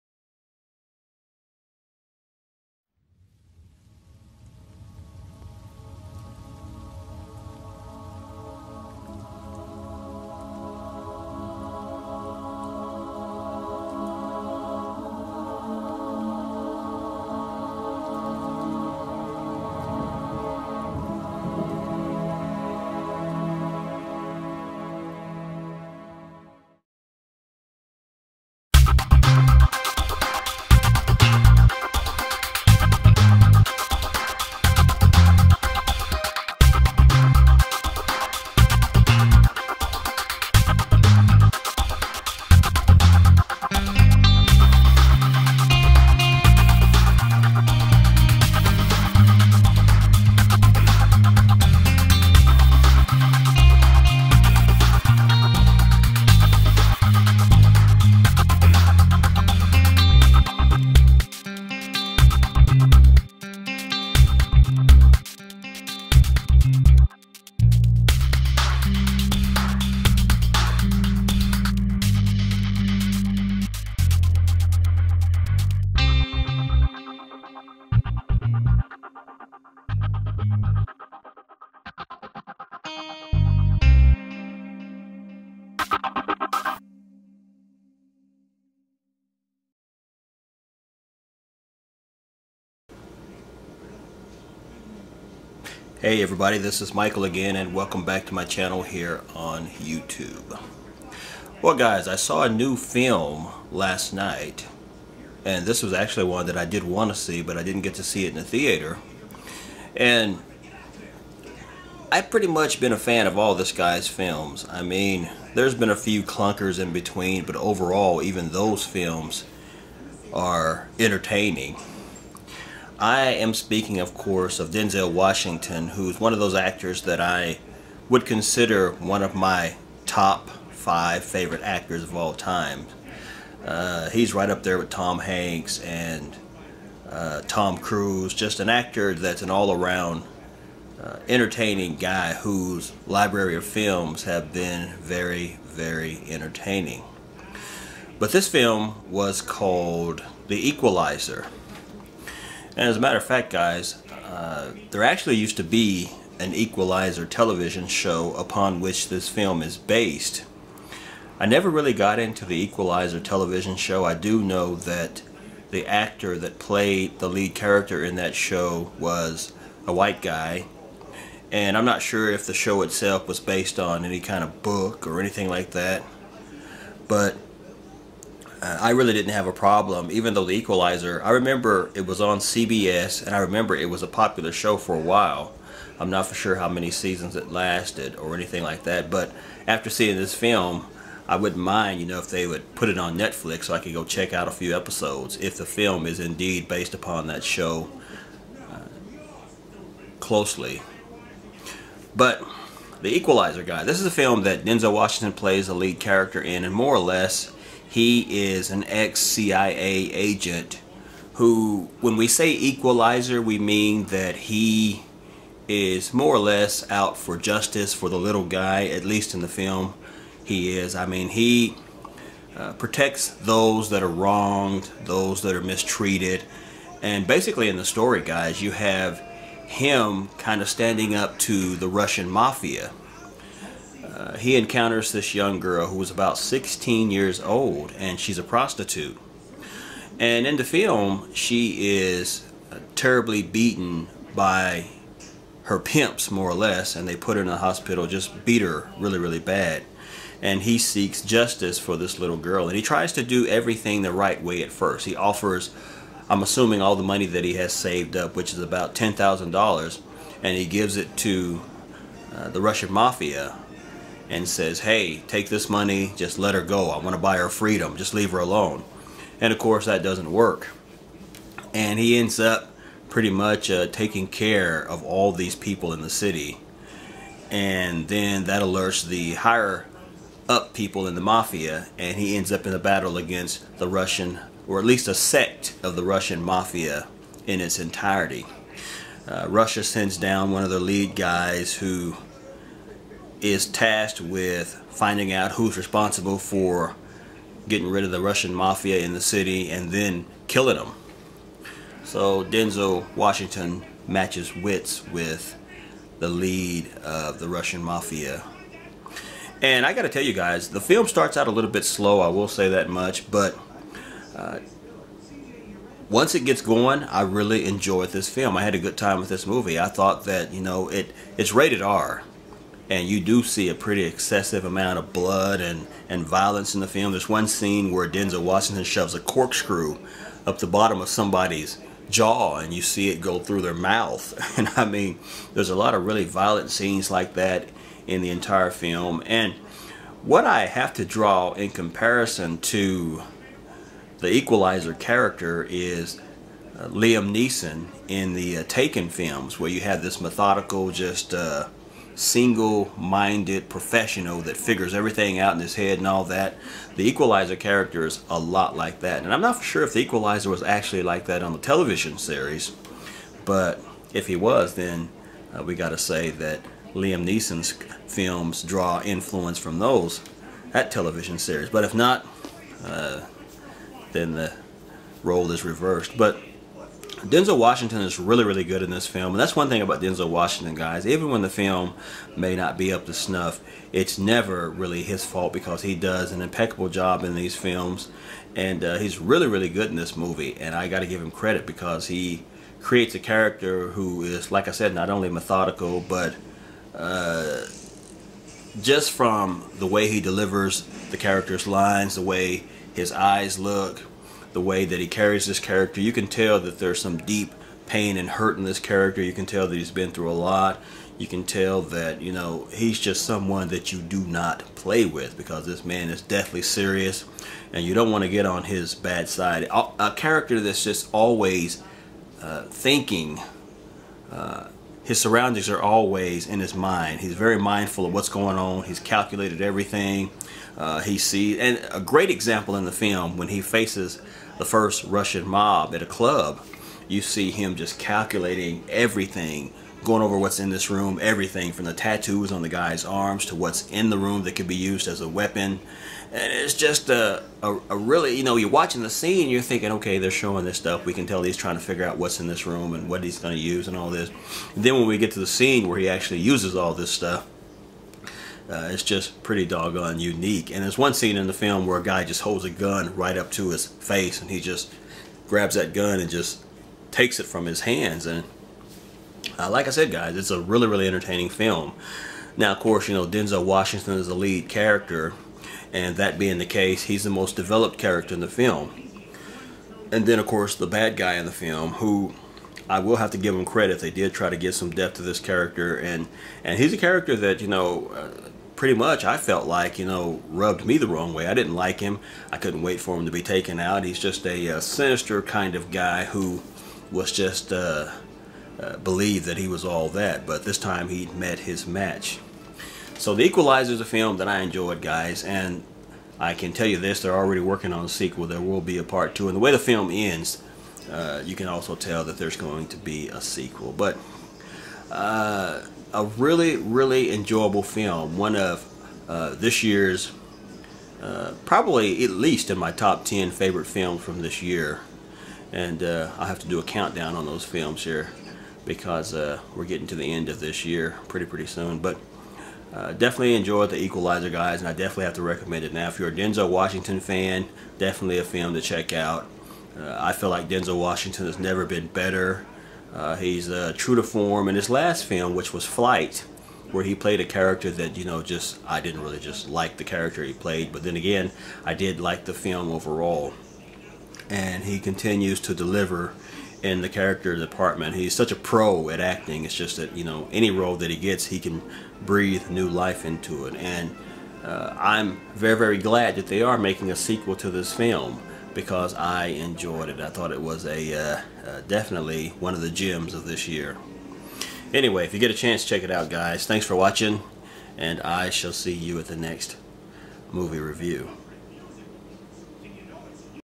TMR - The Equalizer (2014)
Here's a review that got lost in the shuffle...that's why there's still Christmas decorations in the background...

The Equalizer is a 2014 American action thriller film directed by Antoine Fuqua and written by Richard Wenk, nominally based on the television series of the same name. It stars Denzel Washington, Marton Csokas, Chloë Grace Moretz, David Harbour, Bill Pullman and Melissa Leo.

Principal photography began in June 2013 on location and took place in different cities of Massachusetts. This was the first film to have Village Roadshow Pictures co-finance the deal with Sony Pictures Entertainment since Saving Silverman in 2001. The film was premiered at 2014 Toronto International Film Festival on September 7, 2014, and released worldwide on September 26.

The film received mixed to positive critical review and earned a box office gross over $192 million.